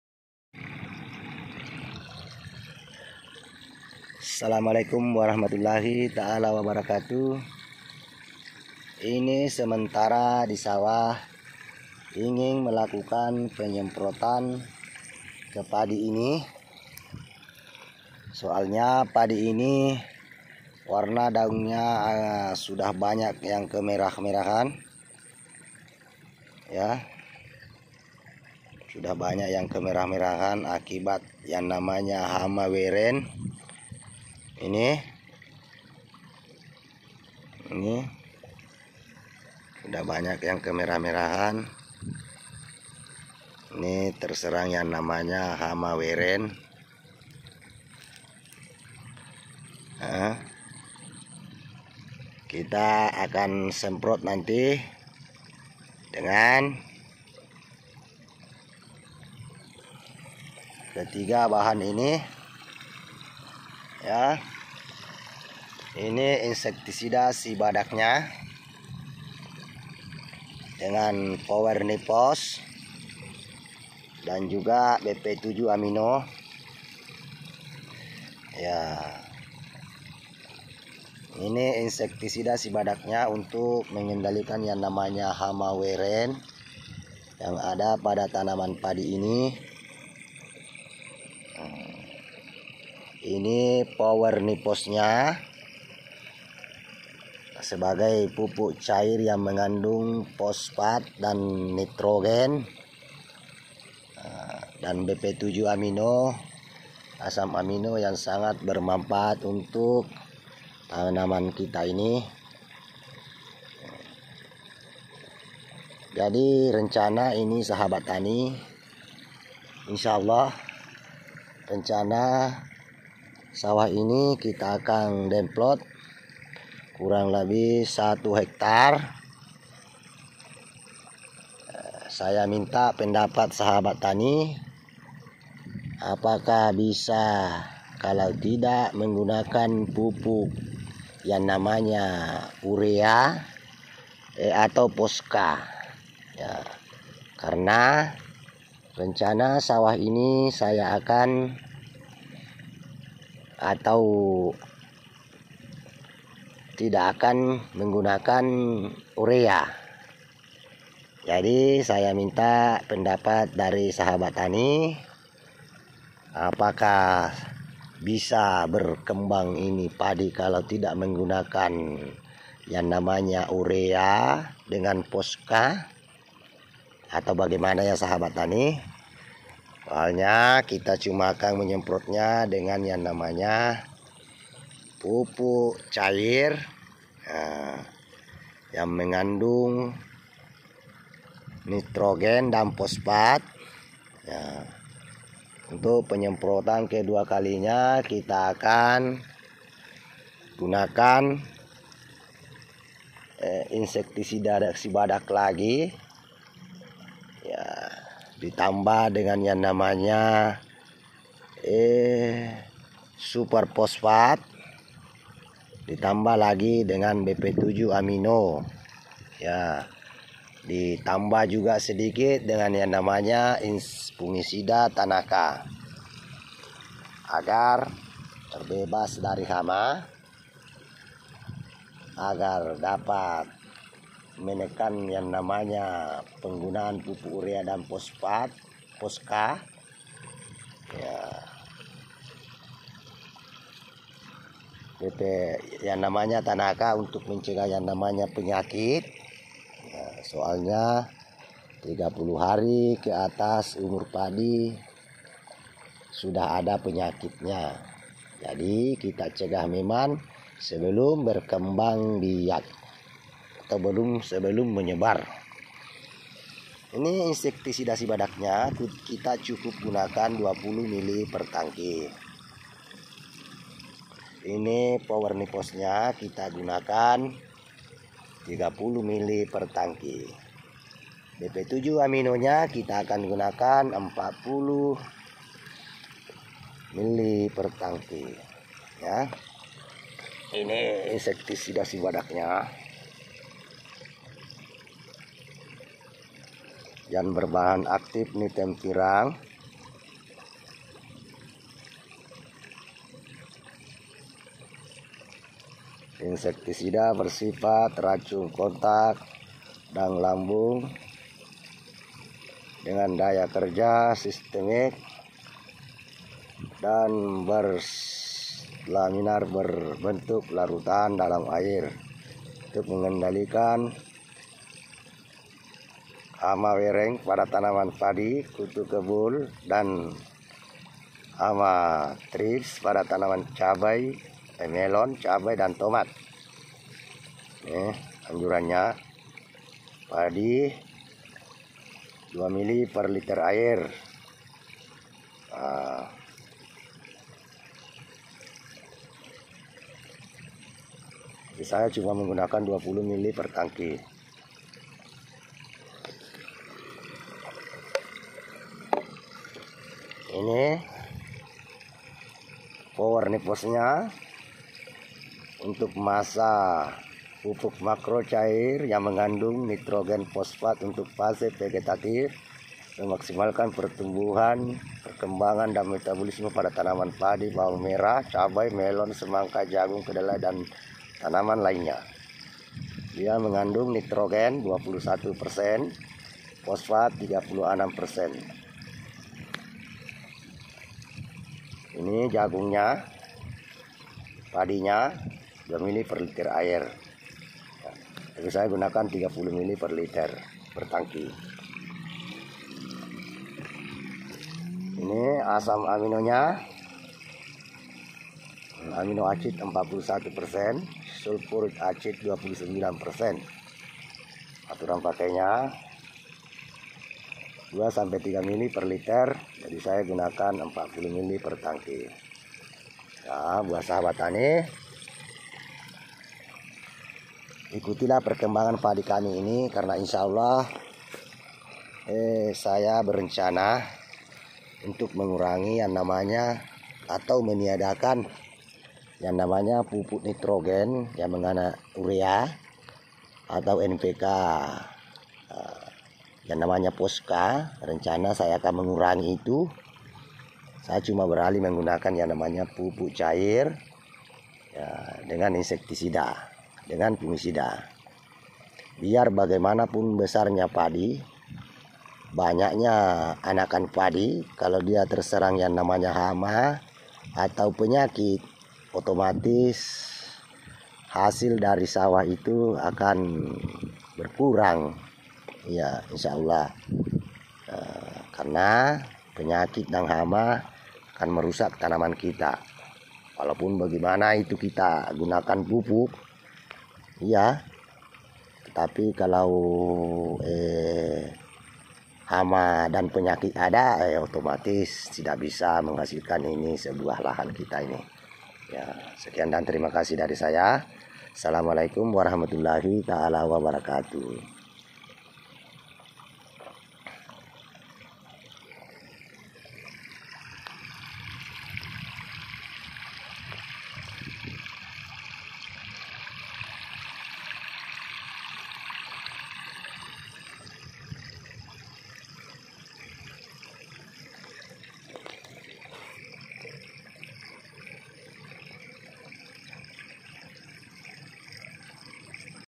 Pengaplikasian insektisida sibadak pada tanaman padi/ part 1
Pengaplikasian insektisida sibadak pada tanaman padi

dosis cuma 20 milli pertangki

gunakan produk kami insyaallah akan mengatasi hama yang ada di tanaman nya